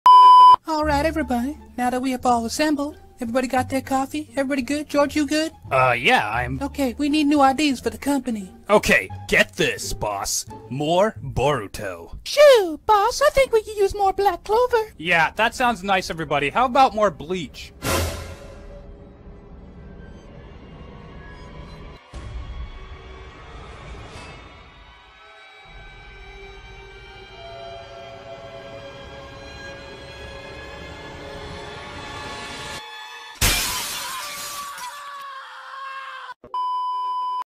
BLEACH SKIT Parody - Shueisha's Plan
THE FOLLOWING IS NOT TO OFFEND ANY OTHER ANIME COMMUNITIES. THIS IS JUST FOR LAUGHS. Do not take it seriously. I do not hate any other series that Shueisha is involved with :D This is just a short skit i did in the past, and i wanted to share it as a separate video! Enjoy! (Yes, the voices are done by me)